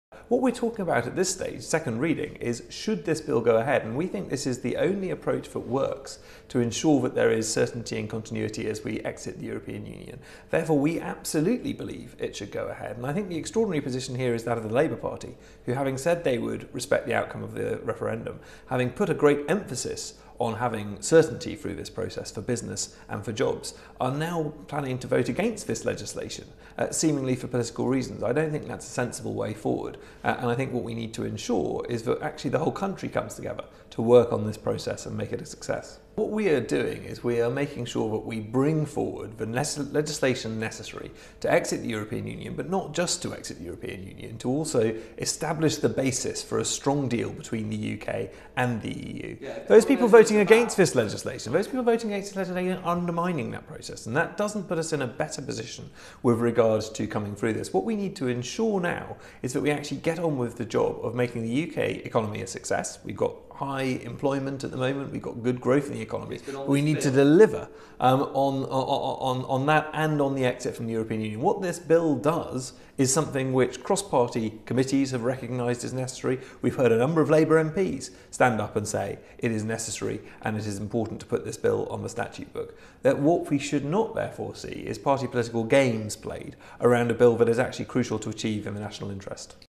Brexit Minister Walker accuses Labour of playing "political games" - Daily Mail
The Scottish First Minister urged Labour to join the SNP in a 'new consensus' to bring about a fresh wave of devolution to Holyrood. And she revealed the Scottish Parliament will publish a series of major policy papers demanding more powers on immigration, trade and employment. Miss Sturgeon has hit out at the UK government over its EU repeal bill, describing it as a 'naked power grab' by Westminster.